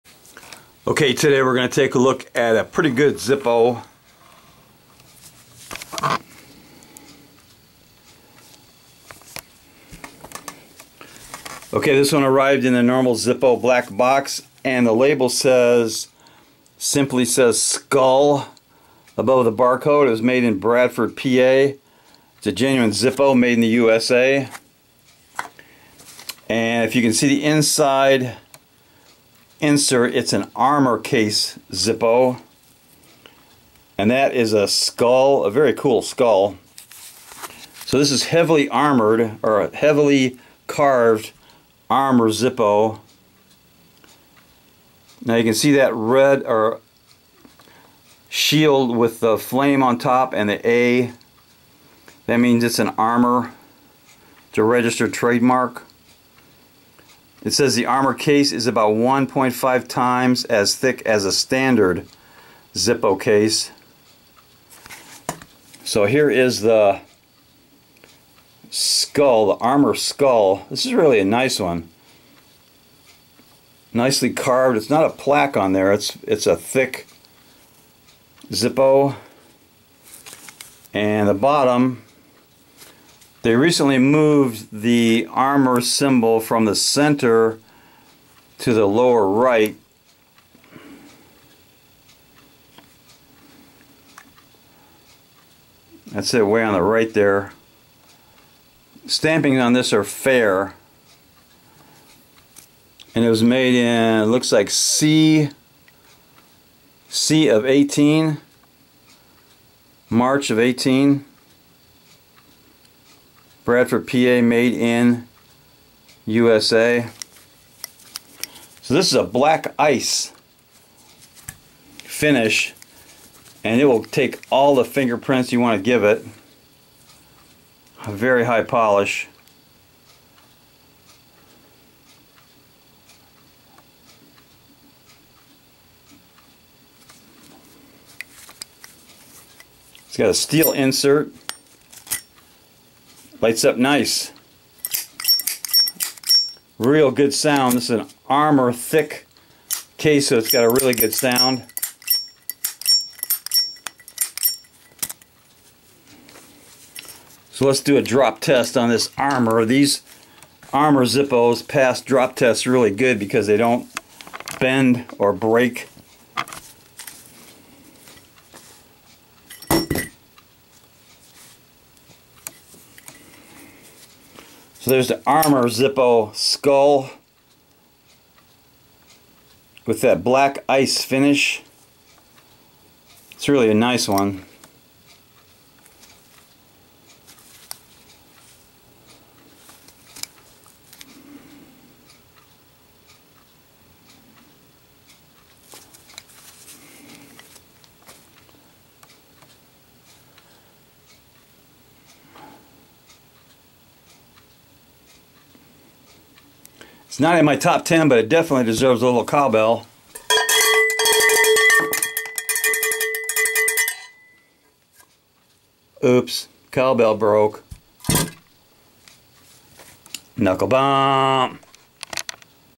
Zippo # Thick Skull, very Thick.
This Armor Skull was made in March of 2018.
It has a High Polish, Black Ice finish. Very Nice.
My CowBell is BROKEN !!!